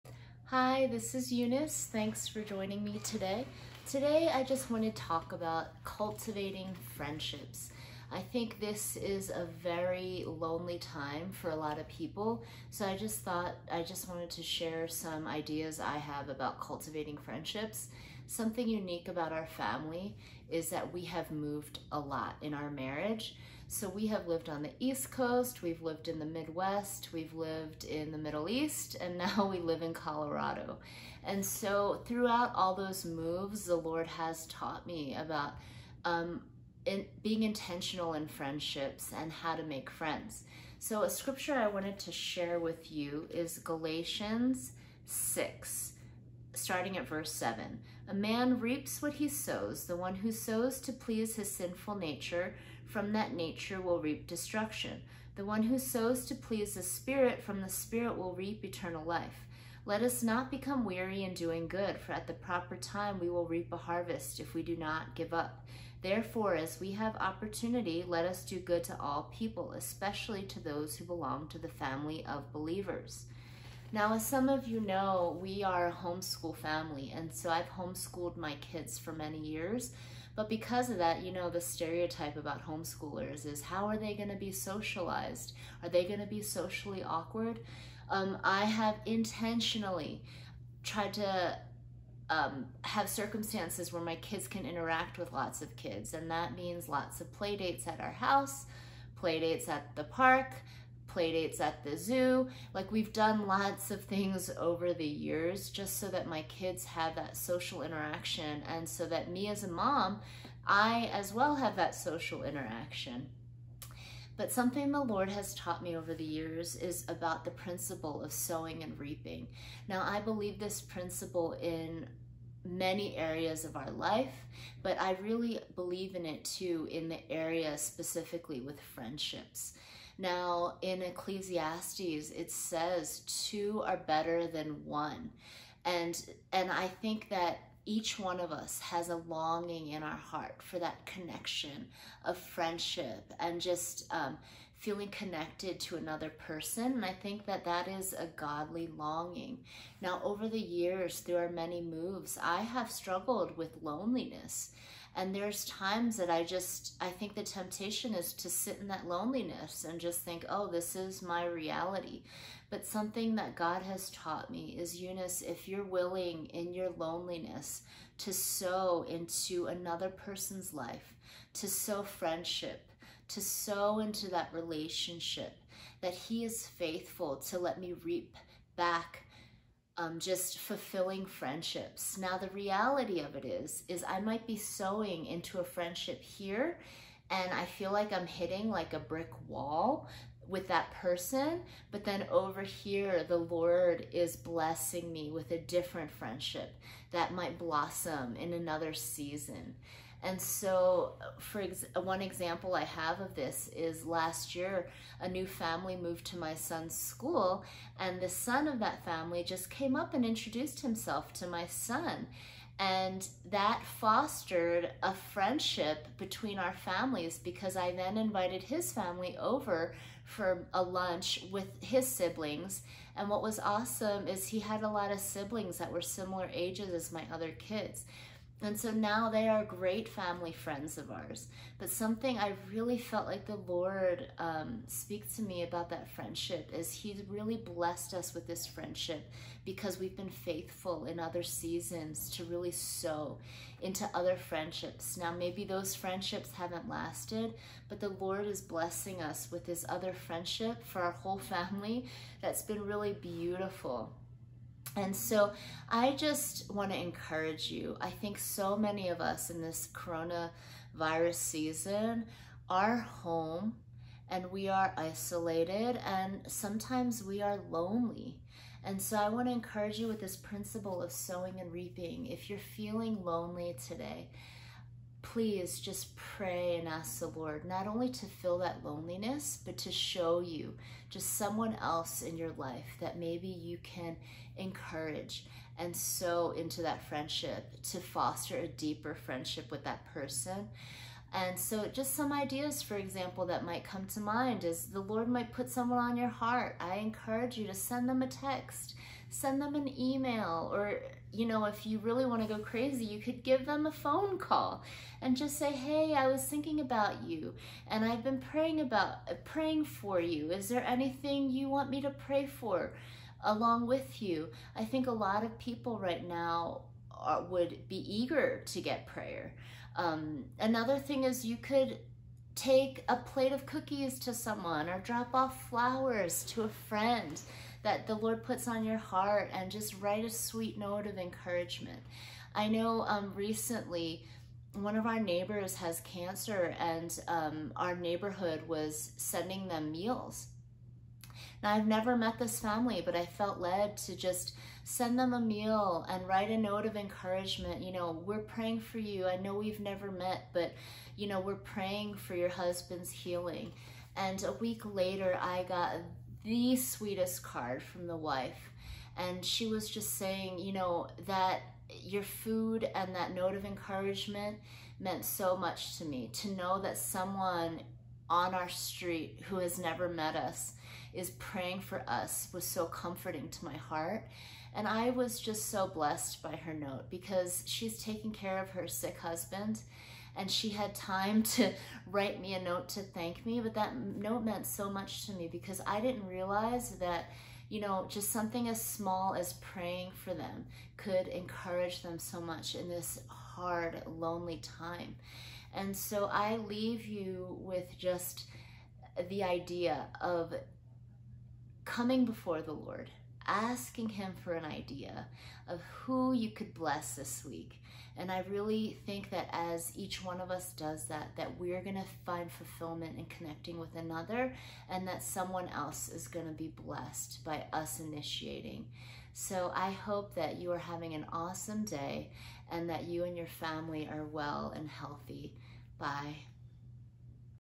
Cultivating Friendships
We've moved a lot in our married life. In this video, I talk about cultivating friendships and something the Lord taught me over the years when I have struggled with loneliness.